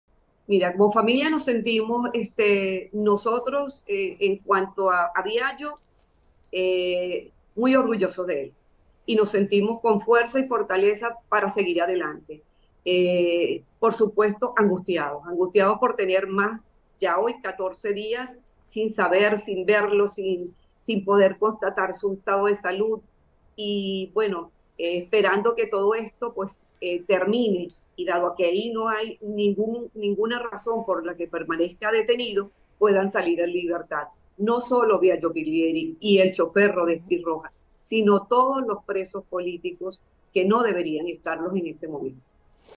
Biagio Pilieri tiene problemas intestinales y de hipertensión, dijo su esposa
María Livia Vasile, esposa del coordinador nacional de Convergencia, Biagio Pilieri, dijo que este sufre de varias afecciones intestinales, así como de hipertensión, y que les preocupa su estado de salud.

«Biagio tiene un cuadro de salud de varias dolencias, problemas de hipertensión y graves problemas en la parte digestiva. Sufre de colon irritable, desgaste a nivel de ambas rodillas y estamos muy preocupados. Días previos a la actividad, ya venía con quebrantos de salud intestinales», declaró a VPI TV.

Este martes, se cumplen 14 días desde que Biagio Pilieri fue detenido tras salir de una concentración convocada por María Corina Machado en Altamira, en la que se conmemoró un mes de las elecciones presidenciales que ganó Edmundo González Uurrutia, según la oposición democrática.

Vasile dijo que desde ese día, no han tenido ningún contacto con Pilieri. «No hemos recibido una llamado, ni los abogados ni nosotros sabemos nada de él ni de su estado de salud» y enfatizó «Biagio es una persona muy trabajadora, comprometida con lo que es su norte, su objetivo y estaba entregado a esto. Sabíamos que había riesgos, pero no por eso dejaba de querer estar allí y cumplir con lo que se había propuesto».

En este sentido, destacó que siguen luchando cada día para que les permitan acceder a su defensa porque hasta ahora, ni Pilieri ni las otras dos personas que fueron detenidas junto a él, «han tenido el derecho de defenderse».

«Biagio Pilieri es una persona seria, responsable, no entendemos las razones por las que está privado de libertad, pero como su familia nos sentimos muy orgullosos de él, con fuerza y fortaleza para seguir adelante. Sin embargo, angustiados porque son 14 días sin verlo, sin saber de él, y esperamos que todo esto termine. No hay ninguna razón para que permanezca detenido, debe salir al igual que todos los presos políticos».